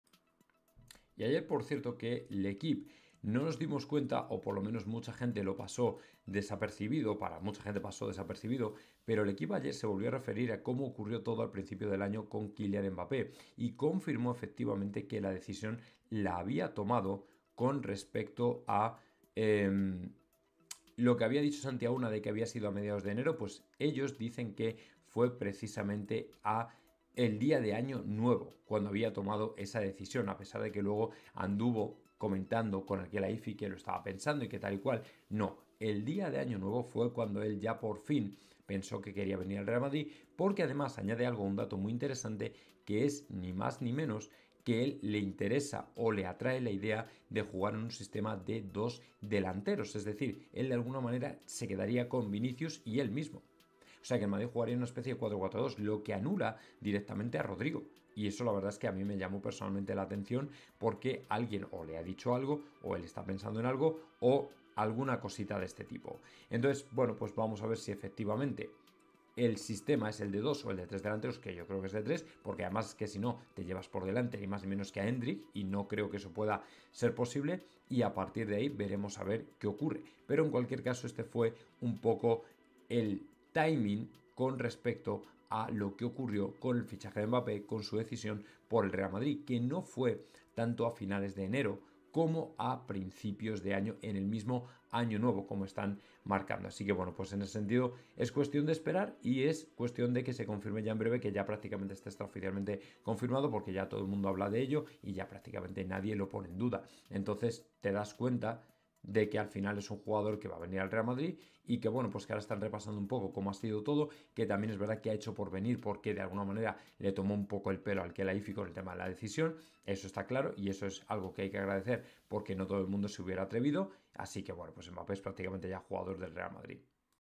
FUE EN AÑO NUEVO | L'equipe desvela cuándo Mbappé TOMÓ LA DECISIÓN y que se la lió a Al Khelaifi
FUE EN AÑO NUEVO  |  L'equipe desvela cuándo Mbappé TOMÓ LA DECISIÓN y confirma que se la lió a Al Khelaifi